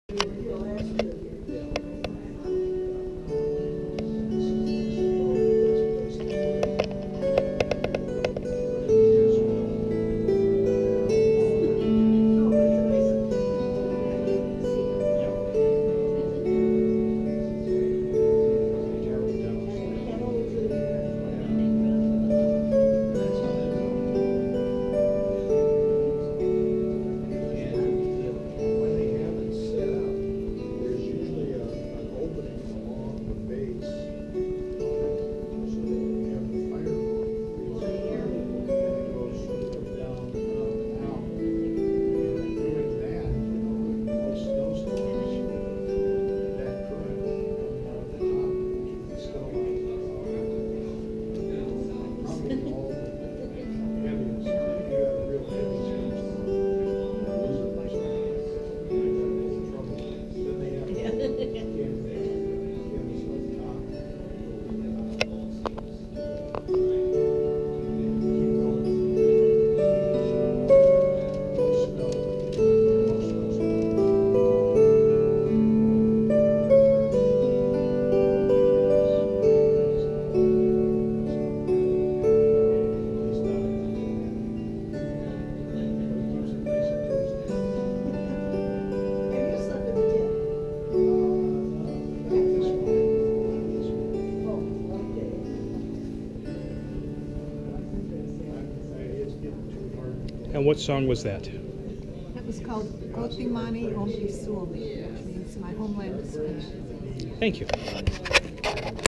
Lee Hoofnagle Playing at Scandinavian Fest 2015
Lee Hoofnagle Playing Traditional Finnish Instruments at Scandinavian Fest 2015 in New Berlin Wisconsin